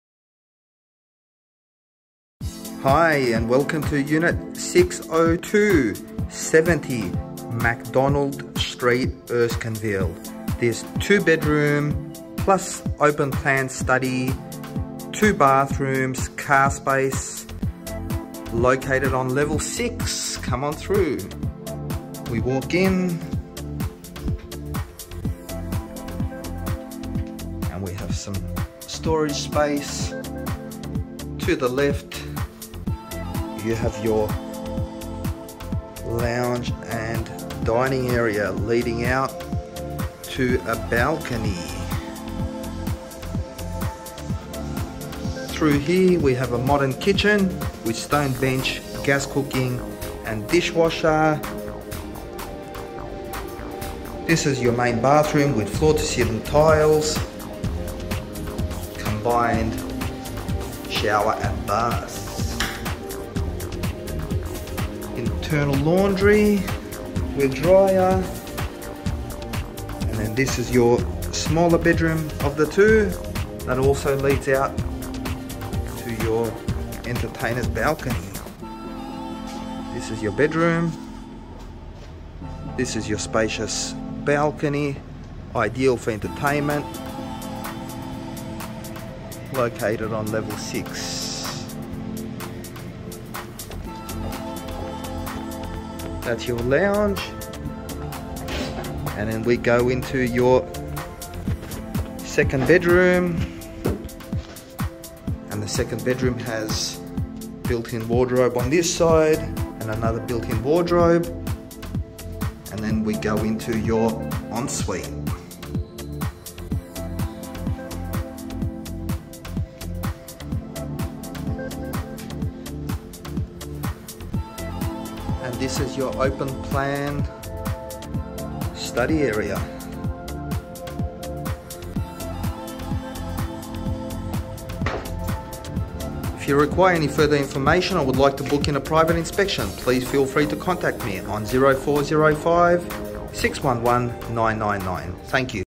FOR LEASE - Modern 2 Bedroom, 2 Bathroom, 1 Car Space Apartment In ERKO Apartments, Erskineville
This modern 2 bedroom apartment is set in a peaceful cul-de-sac street yet so close to everything, it is a short stroll to Erskineville village, train stations, buses, Newtown eateries with Sydney Park plus other amenities just minutes away.

• Two Spacious Sun Filled Bedrooms with built in wardrobes leading out to a balcony
• Study Nook
• Lounge / Dining room leading out to an entertainer’s sun filled balcony
• Two Modern Bathroom with floor to ceiling tiles, Main bedroom with en-suite
• Modern Kitchen with stone bench, gas cooking, dishwasher, and island
• Internal Laundry with Dryer
• Air Conditioning , Intercom , Lift
• Sun Filled entertainer’s balcony with district outlook
• Car Space

For further enquiries please contact our office on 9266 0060

Rent: $740 Per Week
Inspect: As Advertised ONLY
Pets: Will Be Considered With Application